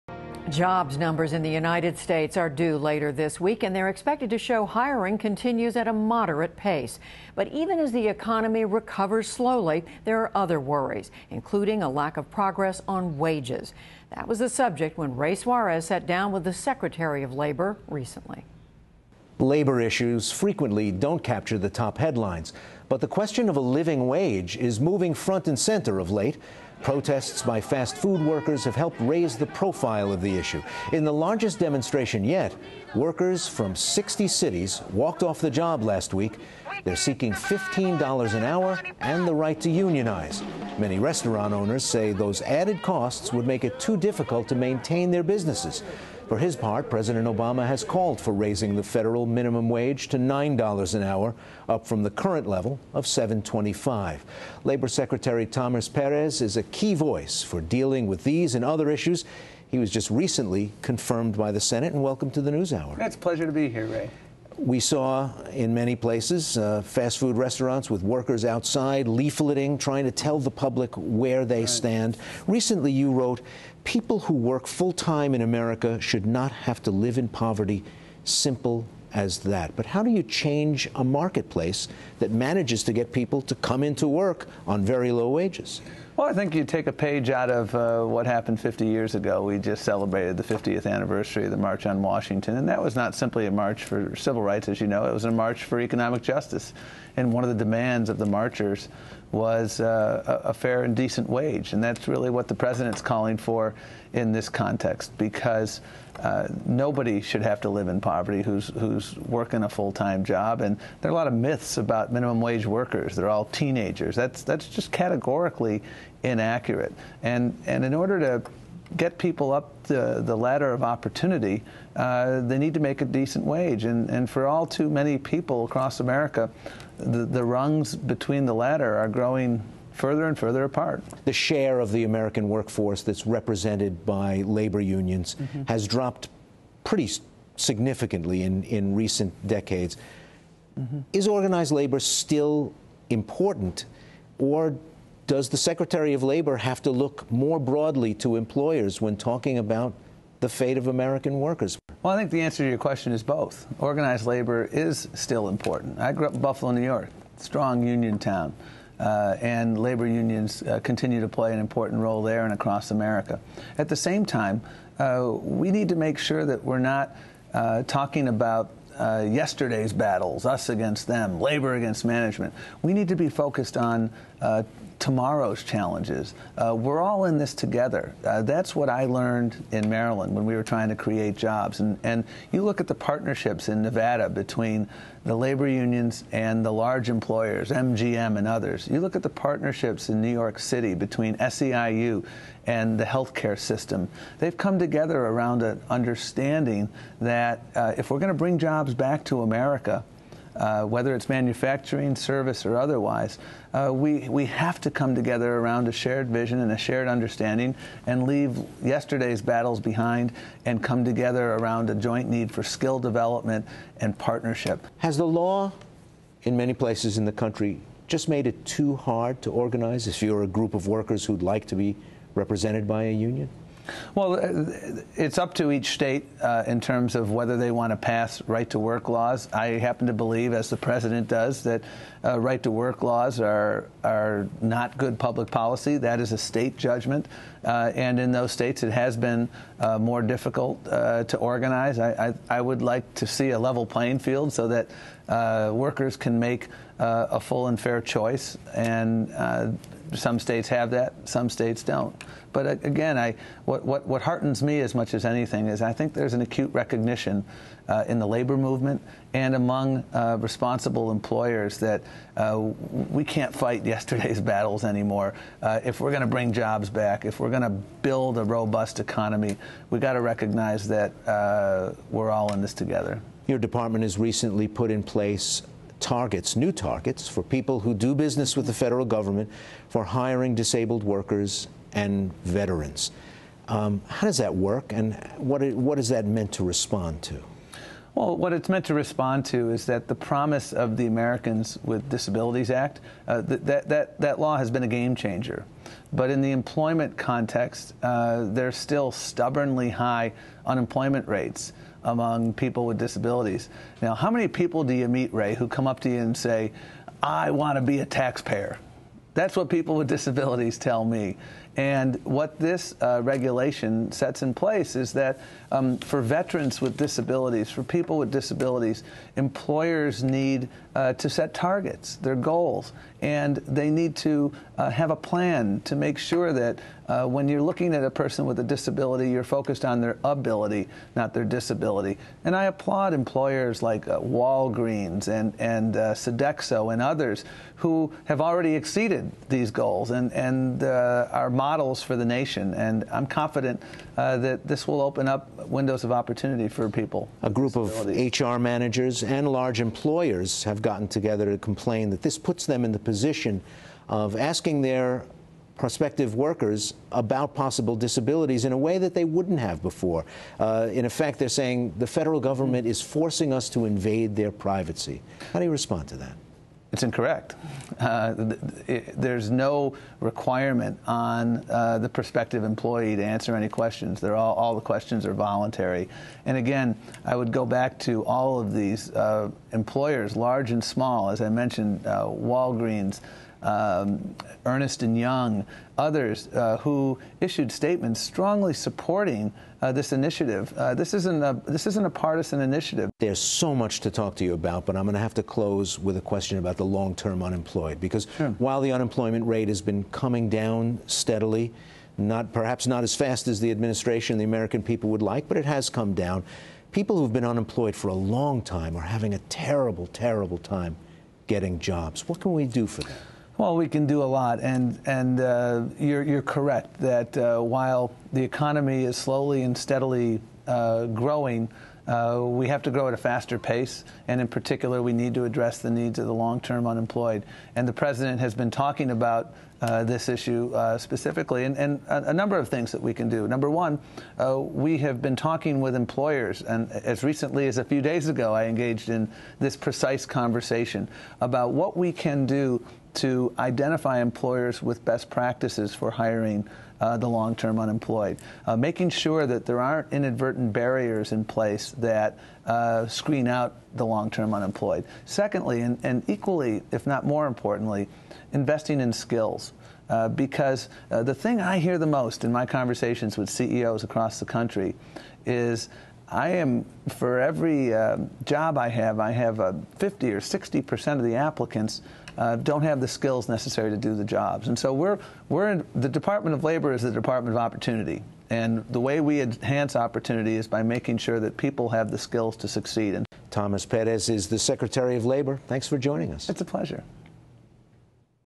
Perez: 'Level Playing Field' Needed for American Workers
Secretary of Labor Thomas Perez believes nobody working a full-time job should live in poverty, but it's hard to make progress when Americans face slow economic recovery and stagnant wages. Ray Suarez sits down with Perez to discuss right-to-work laws, hiring people with disabilities and long-term unemployment.